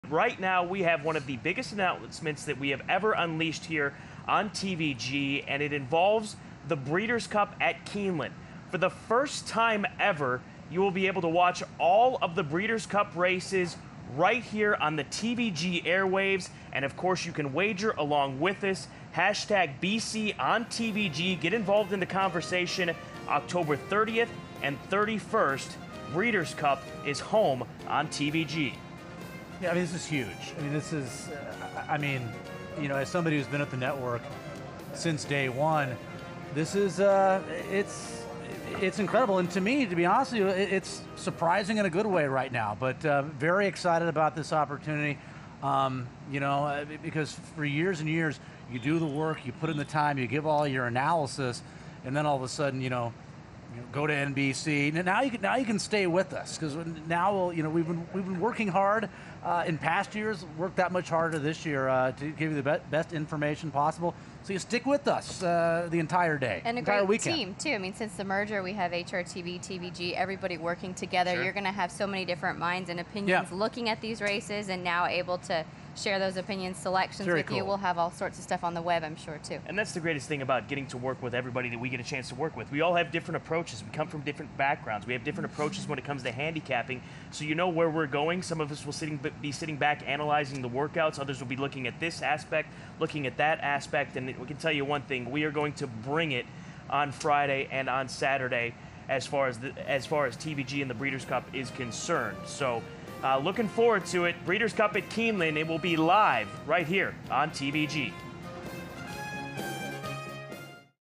TVG to Broadcast 2015 Breeders' Cup
TVG will show live racing from both days of the 2015 Breeders' Cup, which is scheduled for October 30th-31st at Keeneland.